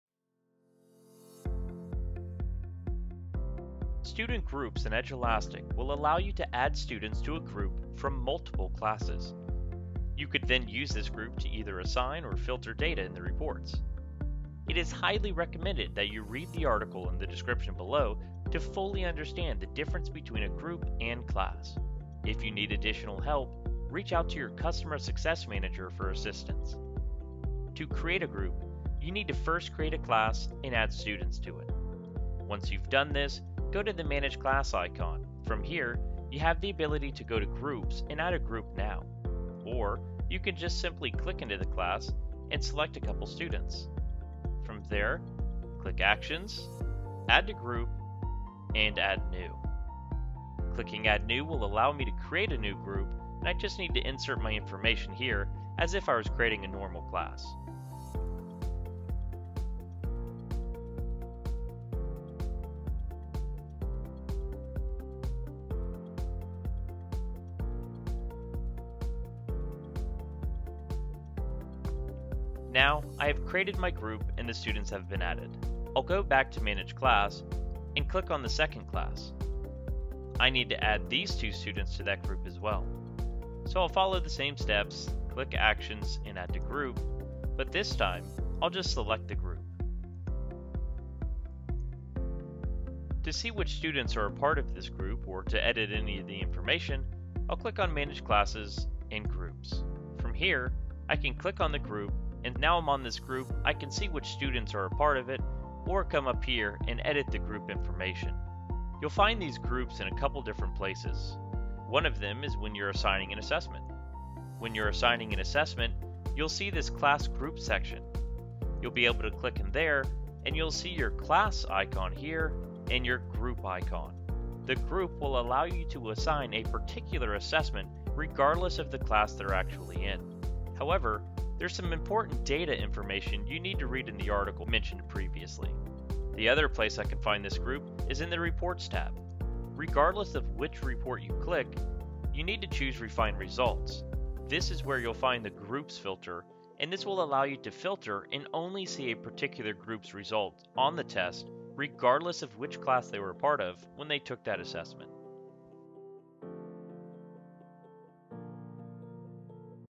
Creating and Using Student Groups
Student groups in Edulastic allow you to add students to a group from several different classes. You can then assign different lessons or assessments for differentiation, or use the groups to dive into data and understand your teaching.

Check this out to learn about the difference between a group and a class: